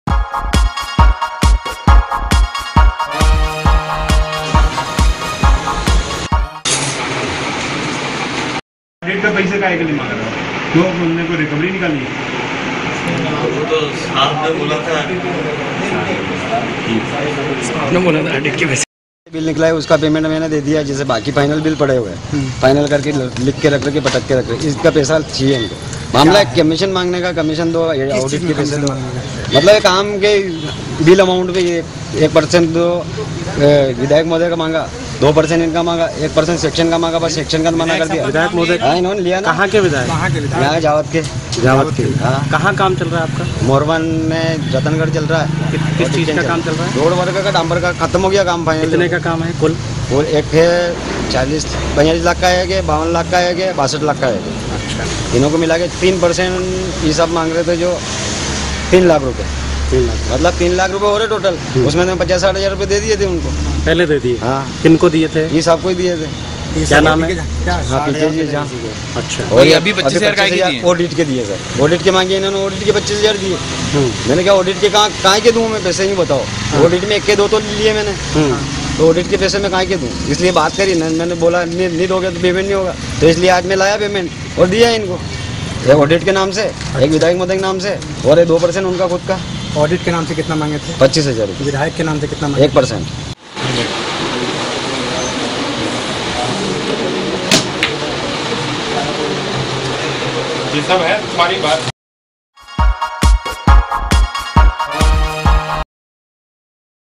भ्रष्टाचार का भंडाफोड़ रंगे हाथ गिरफ्तार
लोकायुक्त उज्जैन की टीम की नीमच में बड़ी कार्रवाई
लोक निर्माण विभाग का वरिष्ठ लिपिक गोपाल जूनवाल रंगेहाथ गिरफ्तार
ठेकेदार से 30 लाख का बिल पास करने के लिए मांग रहा था 25 हजार रुपए
लोकायुक्त उज्जैन के निरीक्षक बसंत श्रीवास्तव के नेतृत्व में हुई कार्रवाई
कार्यपालन यंत्री पीके के झा के खिलाफ भी मामला दर्ज हुआ है